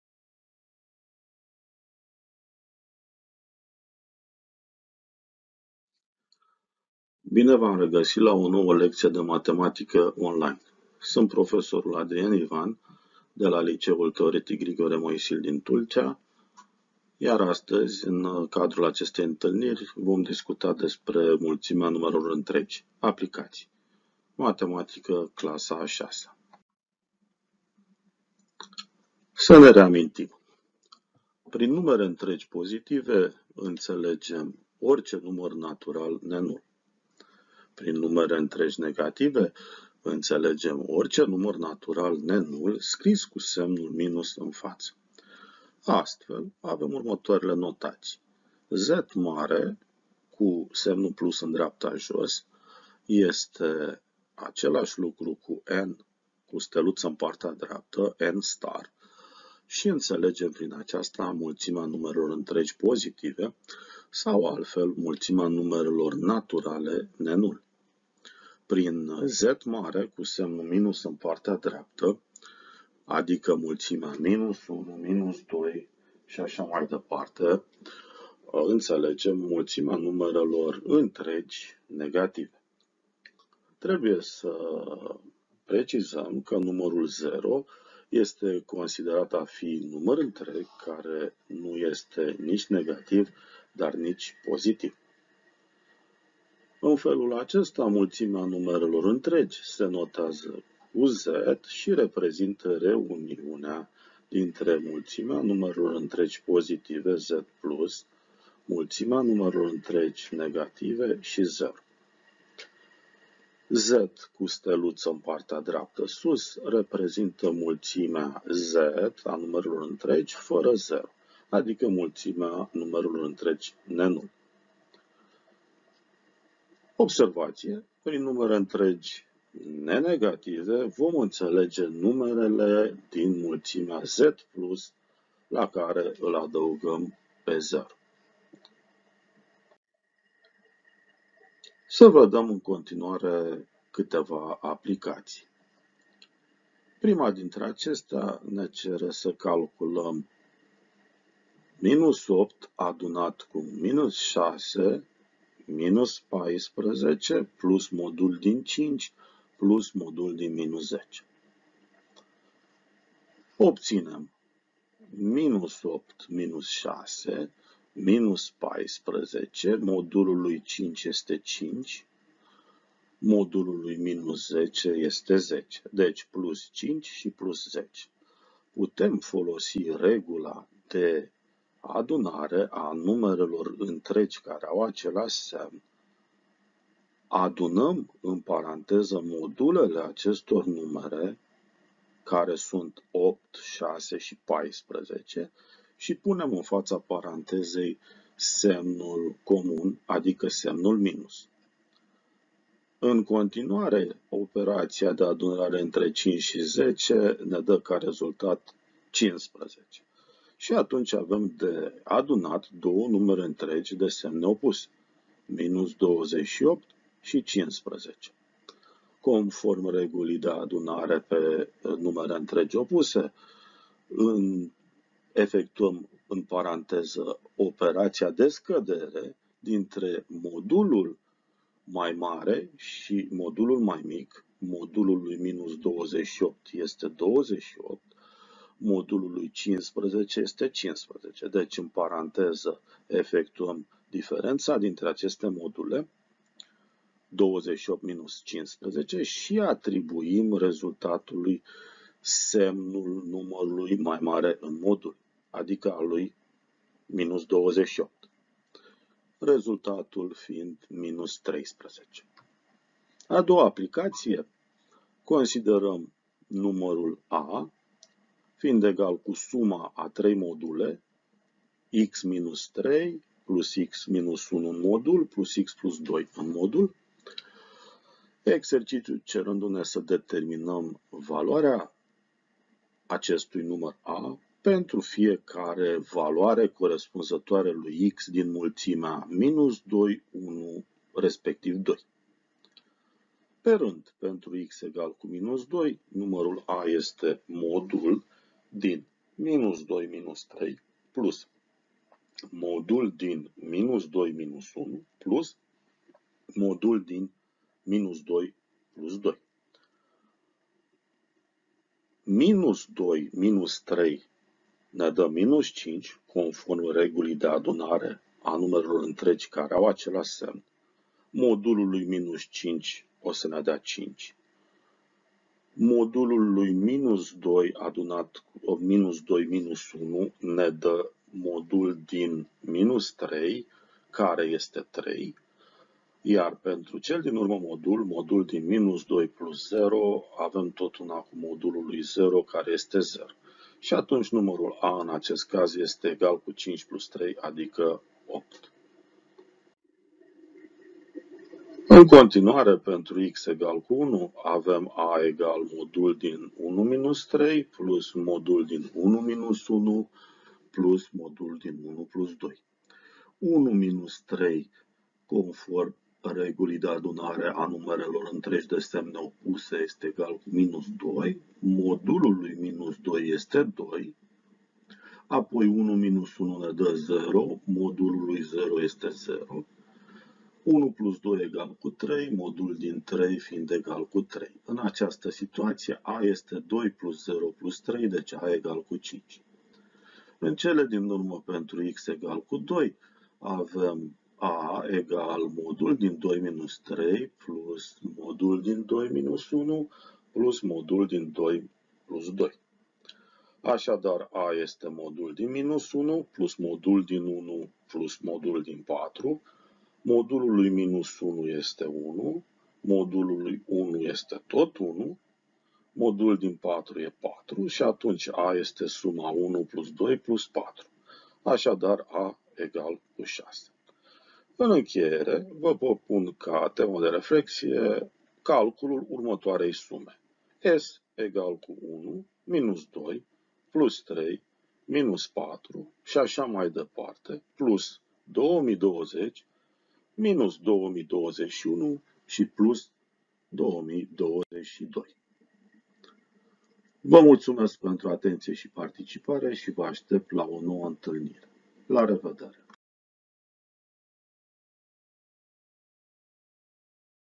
Mulțimea numerelor întregi - Aplicații
Activitatea de învățare asociată (din programa școlară) este aceea de aplicare a unor proprietăţi ale operațiilor cu numere întregi pentru optimizarea calculelor numerice în diverse situații.
Se vor prezenta exemple practice pentru care aplicarea regulilor de calcul şi folosirea parantezelor în efectuarea operaţiilor cu numere întregi sunt esențiale.
Resursa educațională propusă poate să constituie suport pentru cadrele didactice în activitatea la clasă sau online, în lecțiile de predare/recapitulare a noțiunilor legate de numere întregi.
Materialul propus poate fi utilizat singur sau ca parte componentă a unei activități mai ample în cadrul orelor de recapitulare/predare a noțiunilor legate de mulțimea numerelor întregi ( activitate ce presupune atingerea mai multor competențe.

Autor: Adrian Ivan